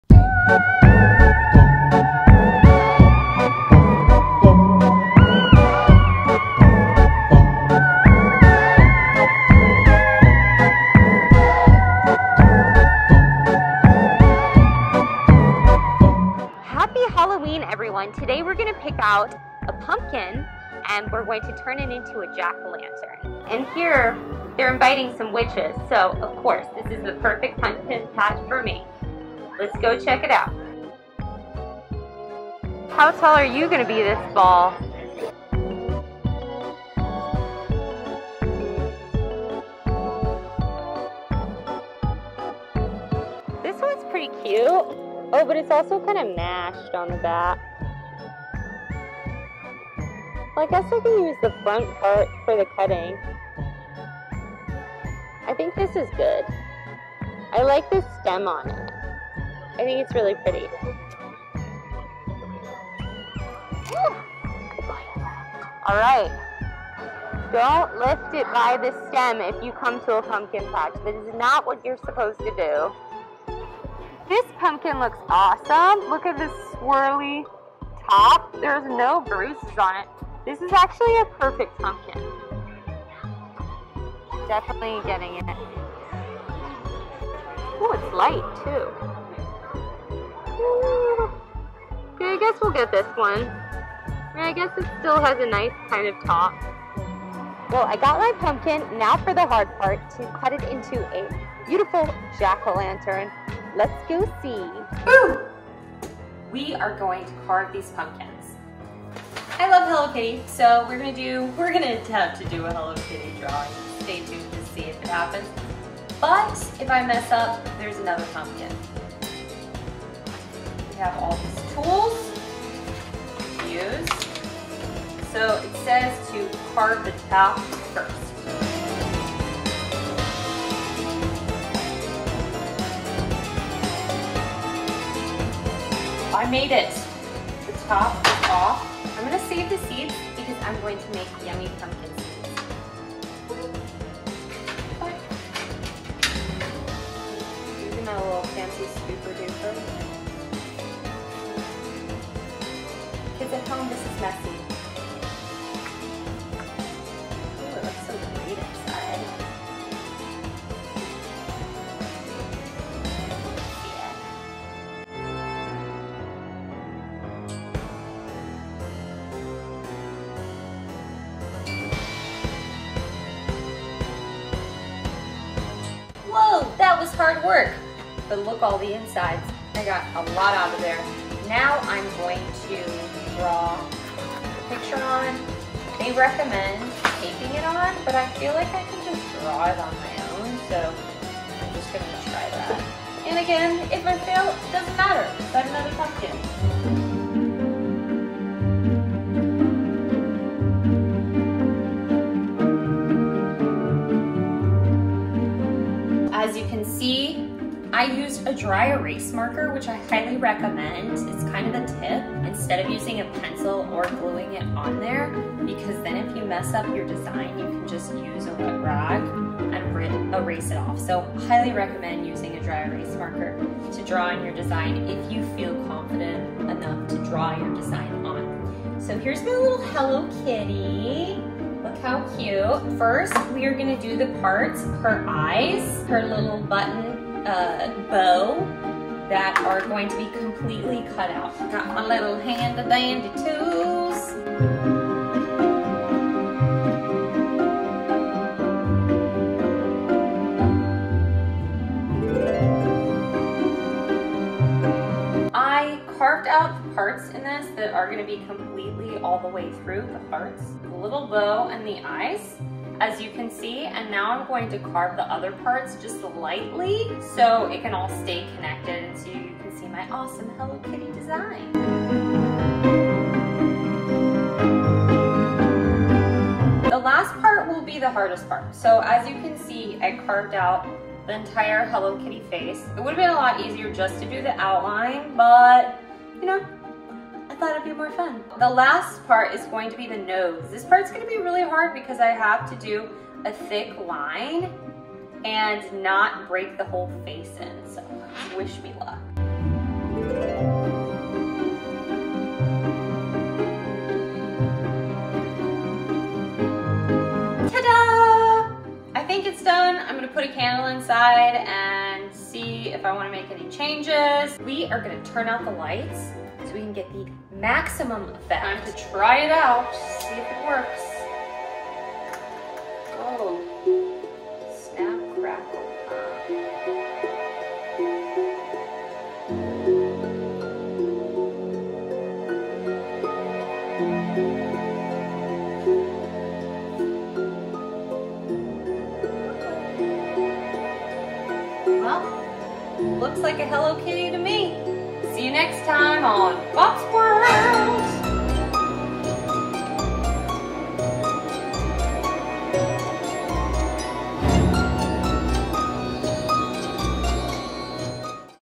Pumpkin Carving technique for Beginners| Boppzworld
💥💥 Pumpkin Carving for Beginners! | Boppzworld 💥💥

I went to a pumpkin patch, picked a perfect pumpkin, and carved the pumpkin!

 💌COMMENT WHAT YOU LIKE🤙👍 OR DISLIKE👎👎 OR WANT TO SEE MORE OF! LIKE THE VIDEO! AND SHARE ON
🌍🌍 REDDIT OR

 OTHER COOL PLACES TO POST VIDEOS!🎬🎬